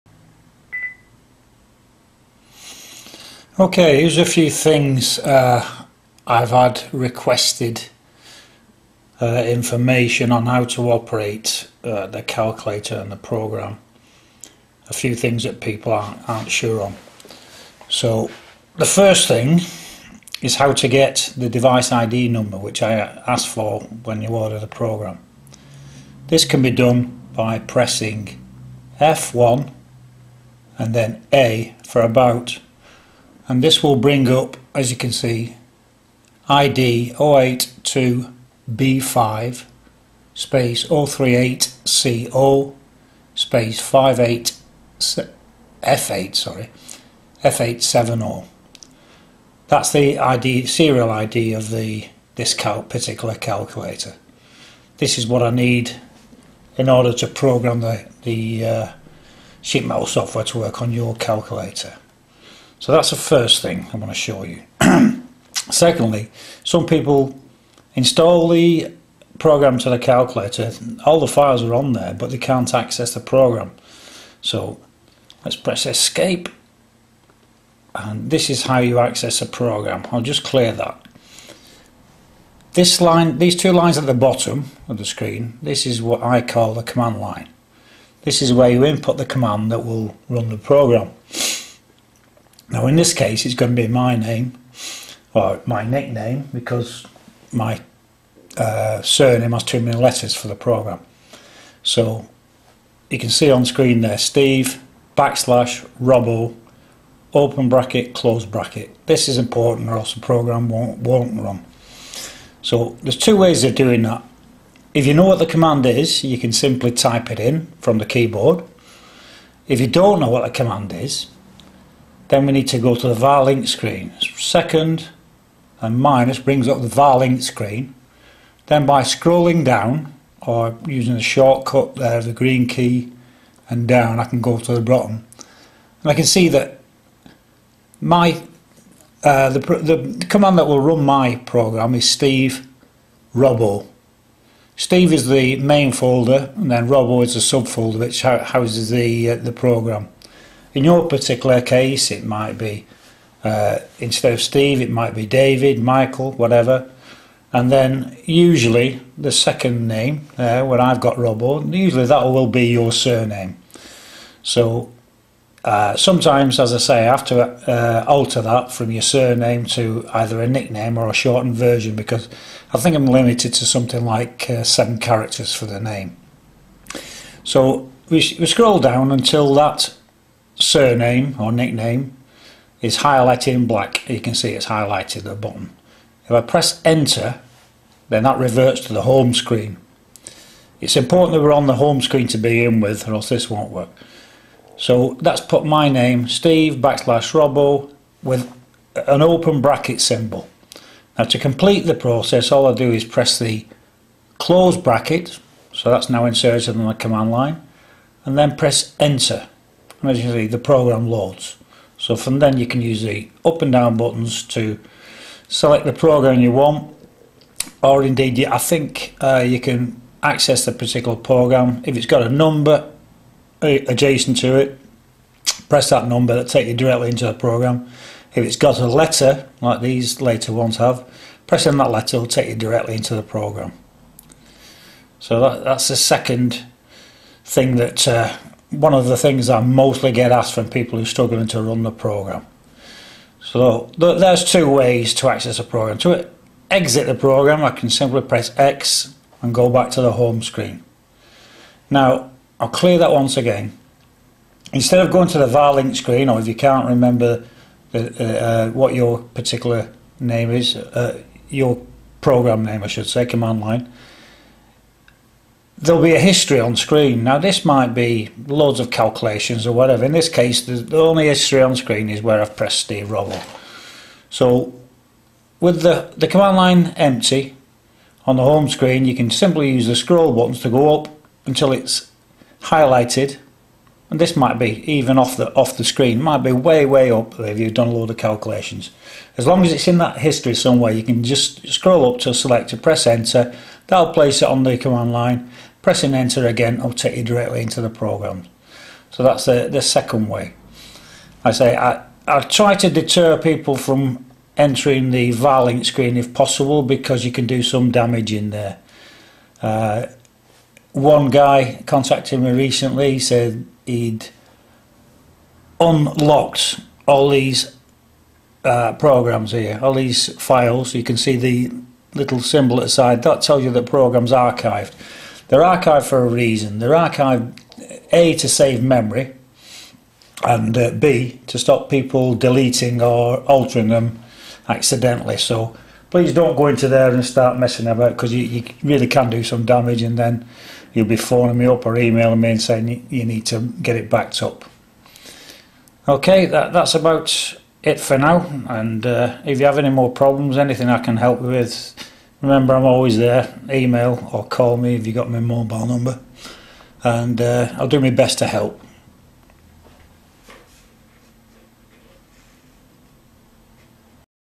TI sheet metal program start up command
A "how to" demo on how to start the sheet metal program once it has been uploaded to your TI calculator.